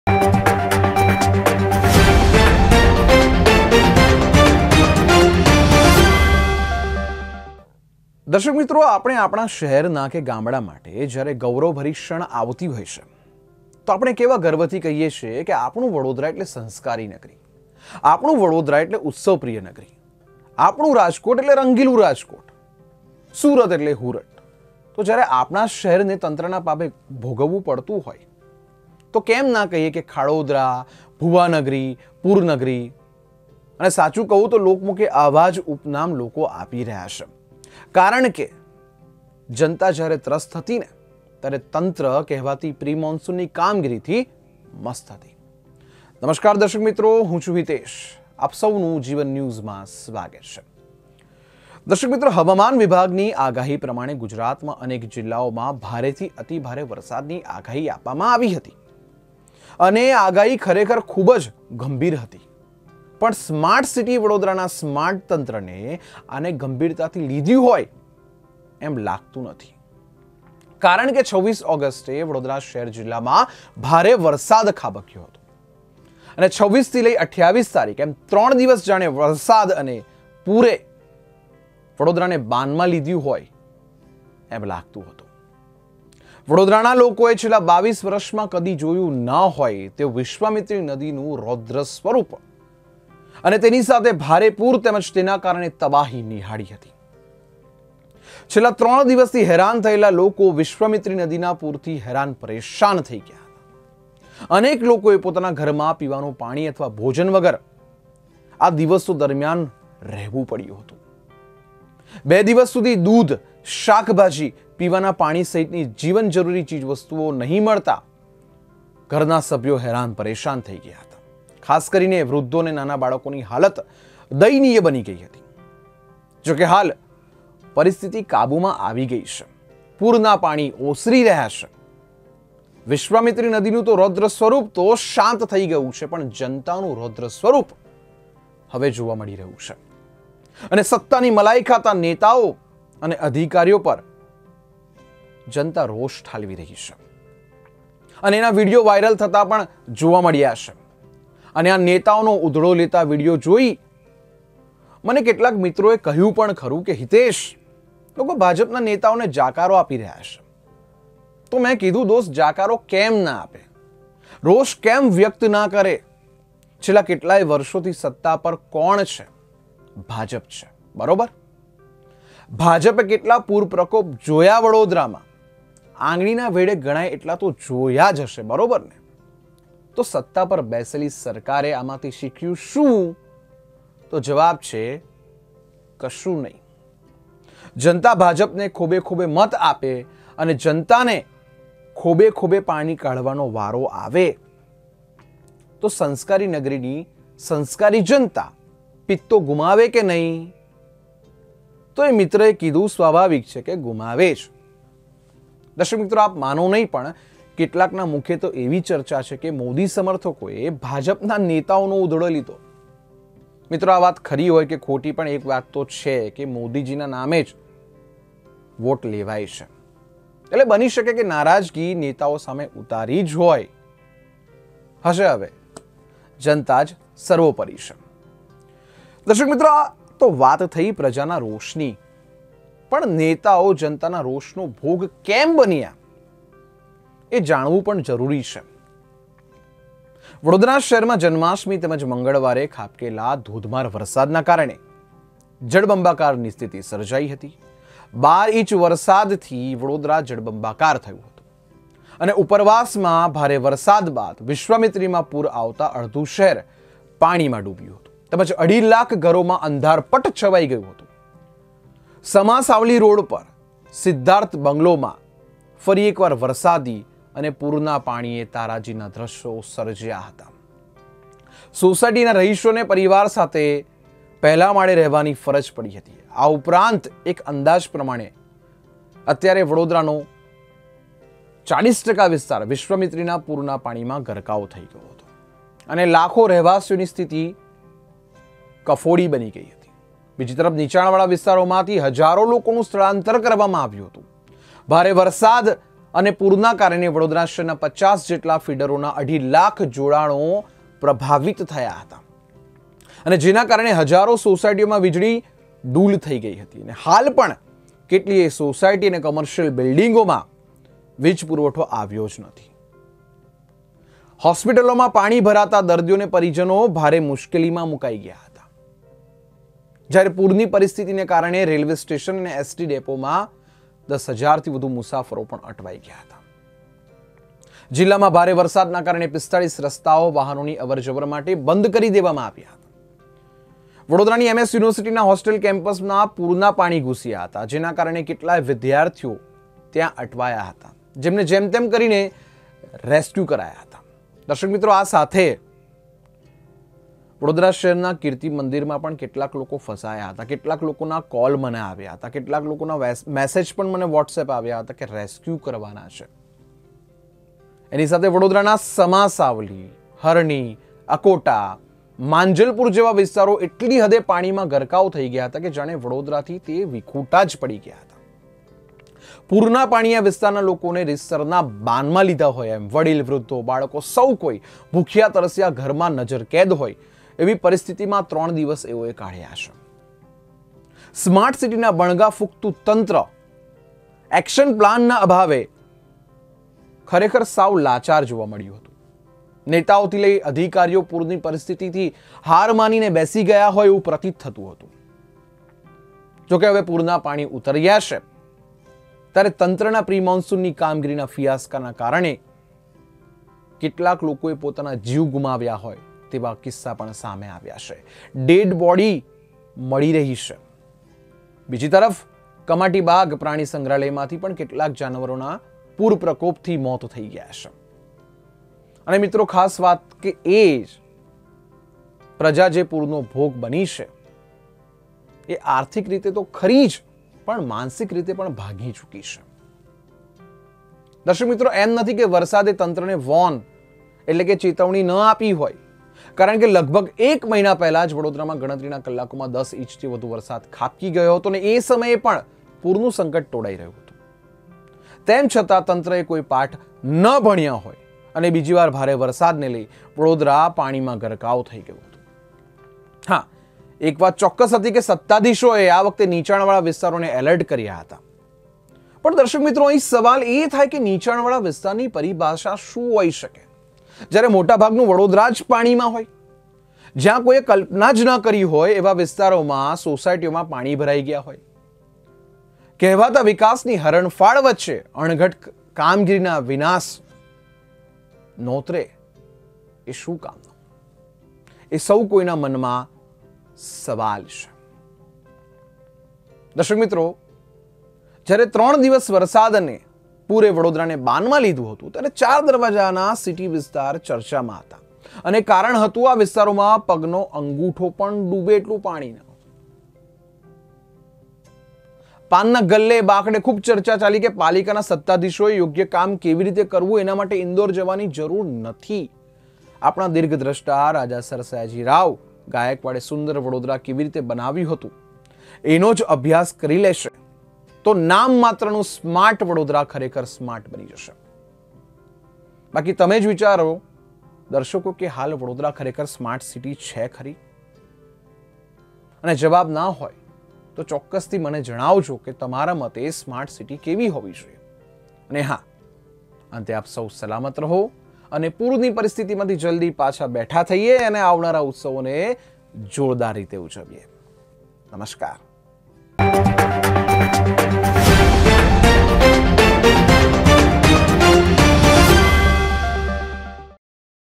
' જળમગ્ન નગરી અને ગંડુ રાજા ' | G One News
દર્શક મિત્રો આપણે આપણા શહેરના કે ગામડા માટે જયારે ગૌરવભરીક્ષણ આવતી હોય છે તો આપણે કેવા ગર્વથી કહીએ છે કે આપણું વડોદરા એટલે સંસ્કારી નગરી આપણું વડોદરા એટલે ઉત્સવ પ્રિય નગરી ... આપણું રાજકોટ એટલે રંગીલુ રાજકોટ ... સુરત એટલે હુરટ તો જયારે આપણા જ શહેરને તંત્રના પાપે ભોગવવું પડતું હોય તો કેમ ના કહીએ કે ... ખાડોદરા ભુવાનગરી પૂરનગરી .. અને સાચ્ચે કહું તો લોકમુખે આવા જ ઉપનામ લોકો આપી રહ્યા છે કારણકે જનતા ત્રસ્ત હતી અને તંત્ર કહેવાતી પ્રિ મોનસુનની કામગીરીથી મસ્ત હતું